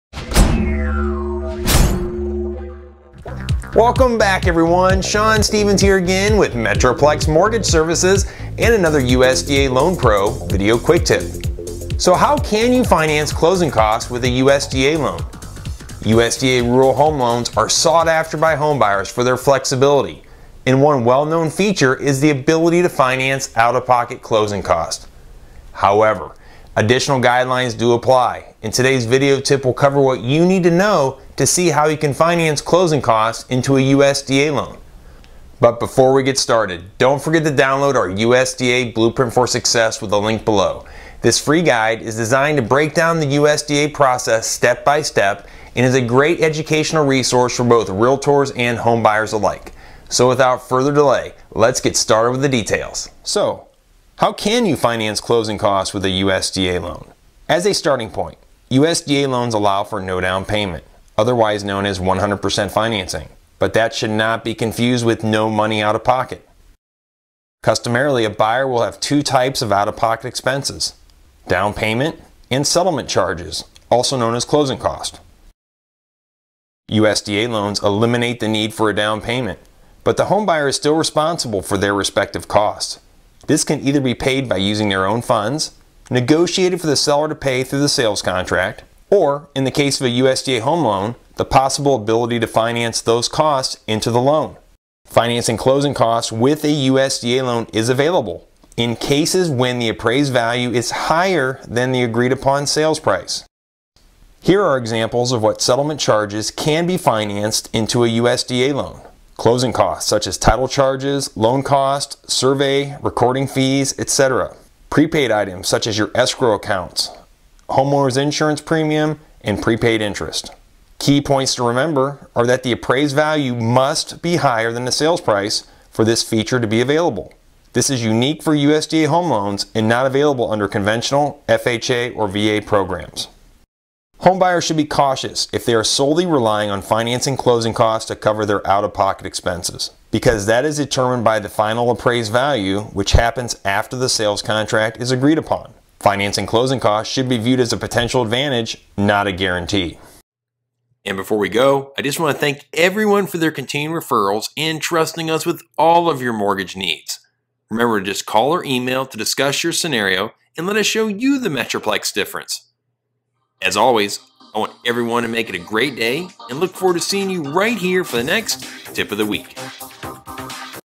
Are closing costs included with a USDA loan?
How Can You Finance Closing Costs With A USDA Loan?

USDA Rural Home loans are sought after by homebuyers for their flexibility and one well known feature is the ability to finance out of pocket costs.

However additional guidelines do apply and today’s video tip will cover what you need to know to see when you can finance closing costs into a USDA loan.

This free guide is designed to break down the process step by step and is a great educational resource for both Realtors and homebuyers alike.

GET MORE INSIGHTS